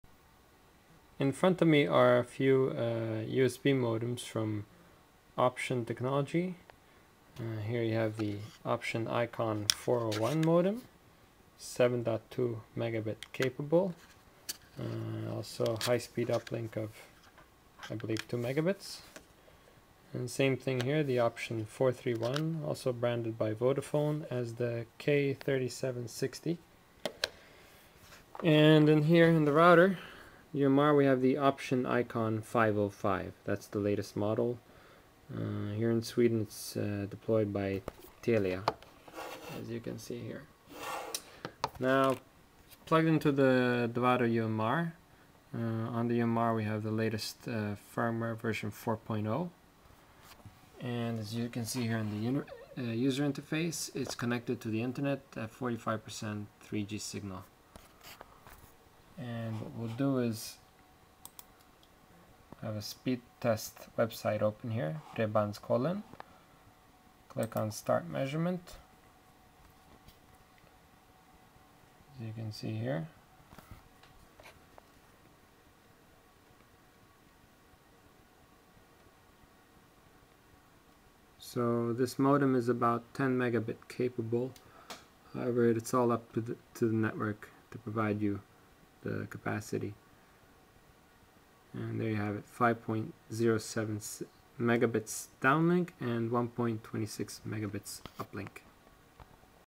DOVADO UMR and Option ICON 401, 431 (Vodafone K3760) and 505 modems
DOVADO USB Mobile Broadband Router (UMR) supporting the latest ICON modems from Option Technology.

Option ICON 401
Option ICON 431 (Vodafone K3760)
Option ICON 505 (up to 10Mbps HSPA+)